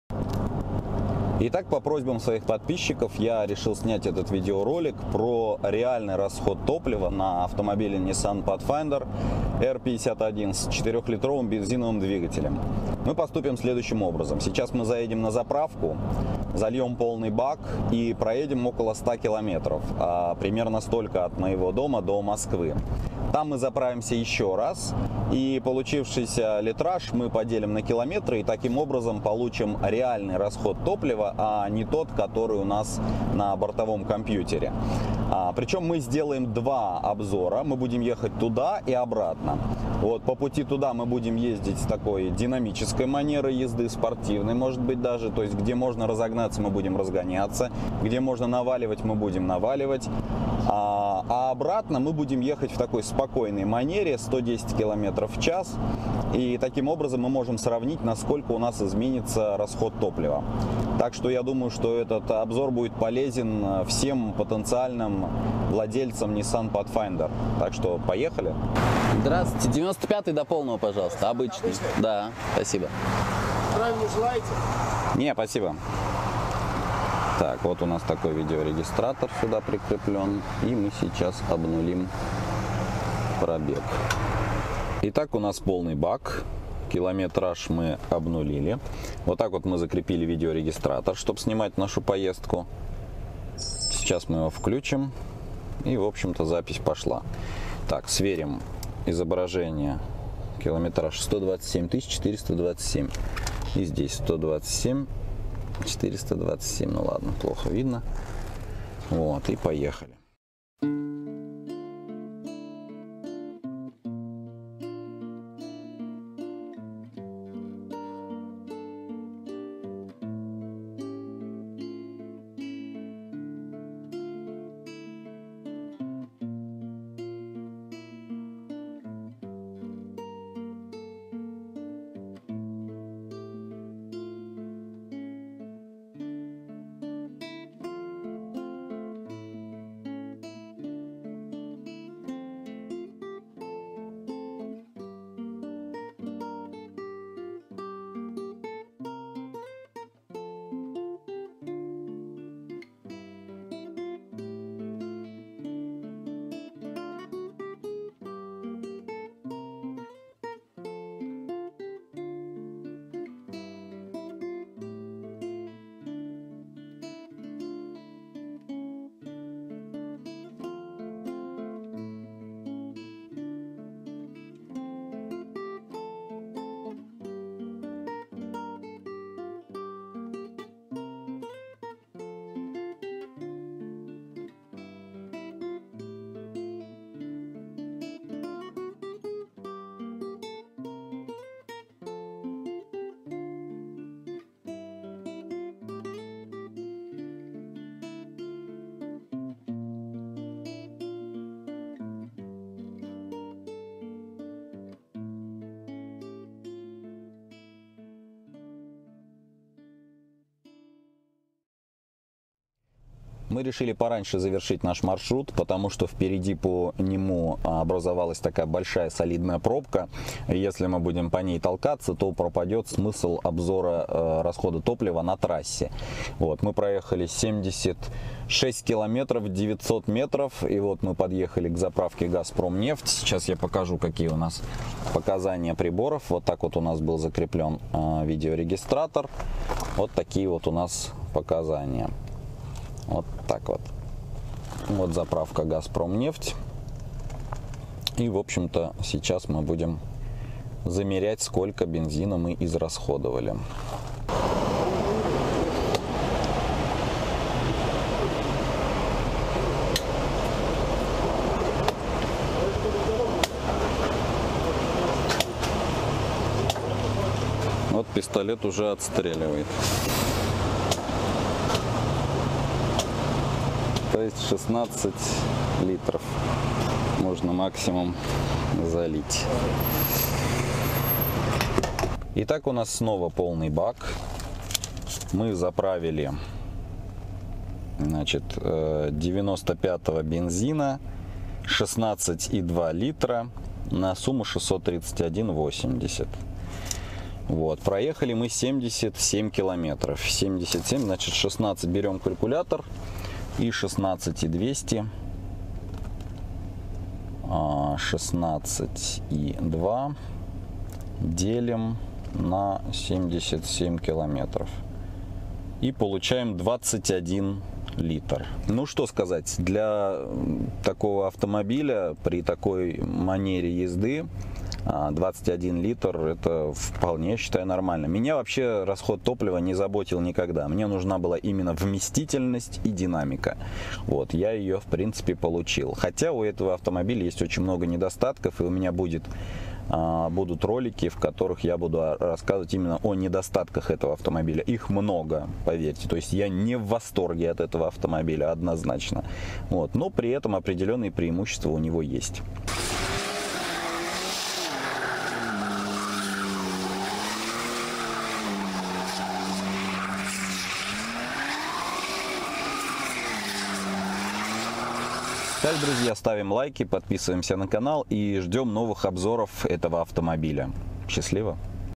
Расход топлива на Nissan Pathfinder при динамичной езде
Самый честный обзор про расход топлива на автомобиле Nissan Pathfinder R51 с бензиновым двигателем 4.0L при спортивной динамичной езде по трассе и городу в смешанном цикле.